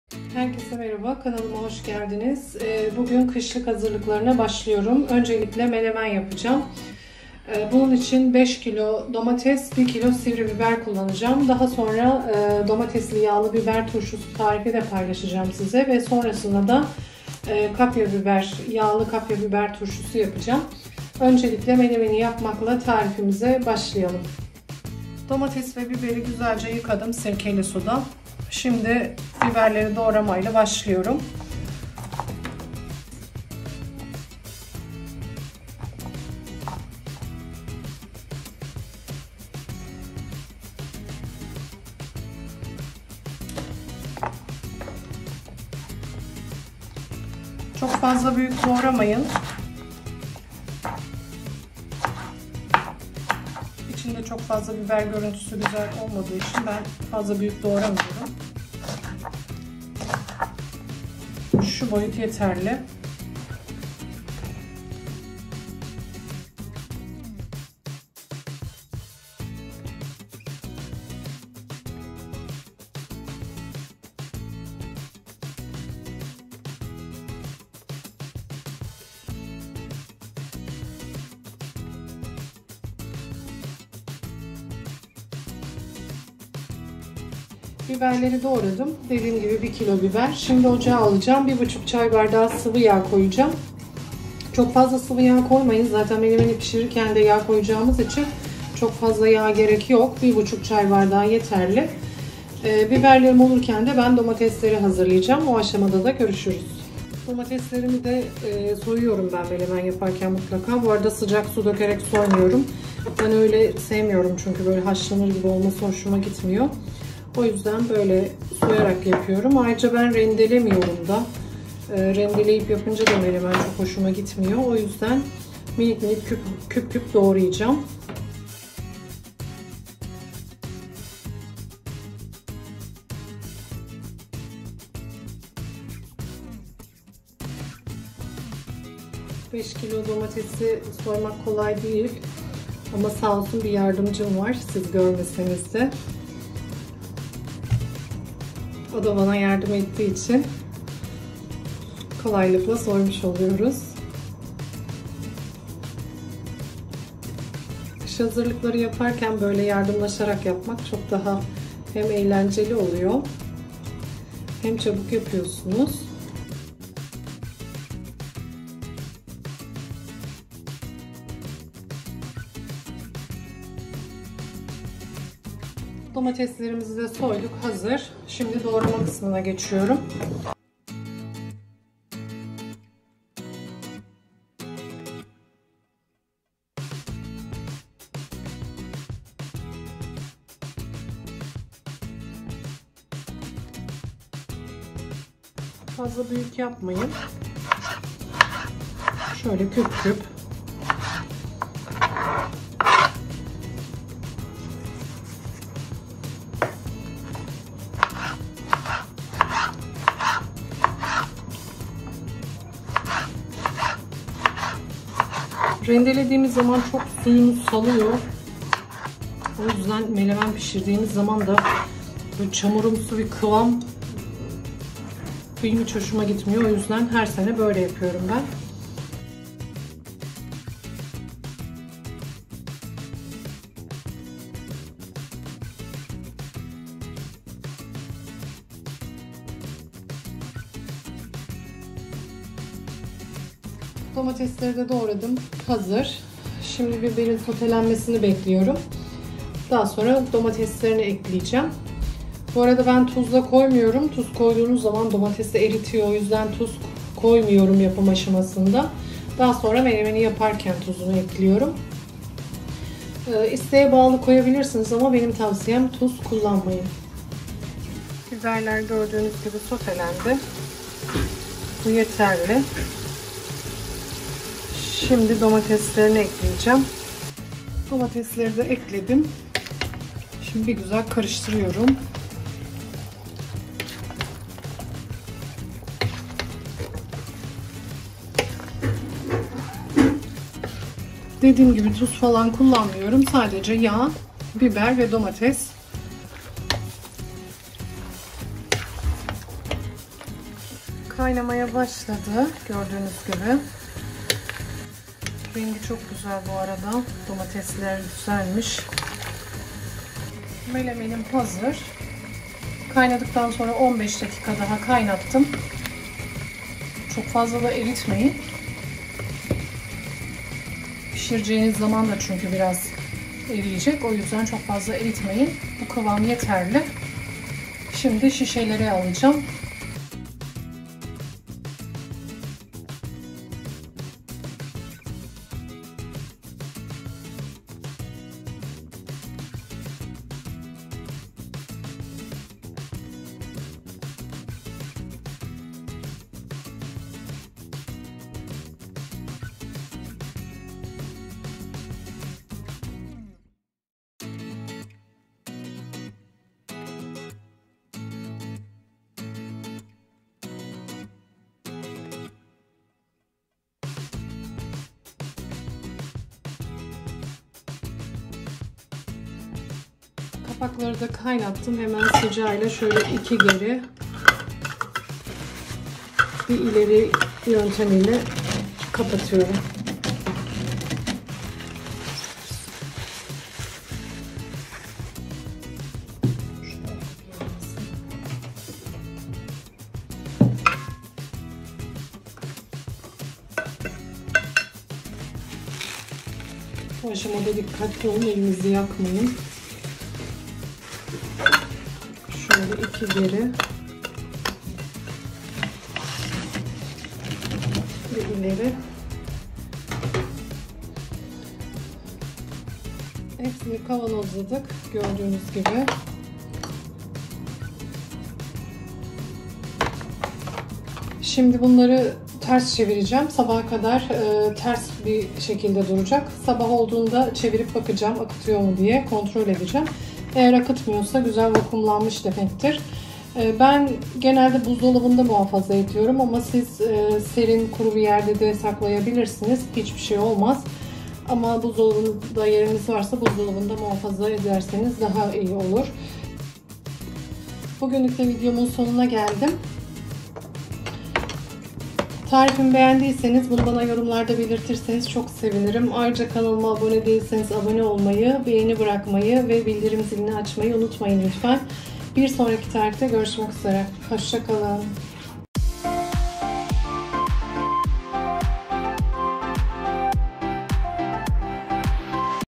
✅ ASLA BOZULMAYAN ‼️ EN LEZZETLİ 💯EVDE KIŞLIK KONSERVE MENEMEN TARİFİ | MUTLAKA DENEYİN 💯
Şimdi ucuza yap kışın rahat rahat ye
Malzemeler :
5 kg domates
1 kg tatlı sivri biber
1,5 çay bardağı sıvıyağ

Tarif videomda ayrıntılı olarak var. Sonuna kadar izleyin lütfen.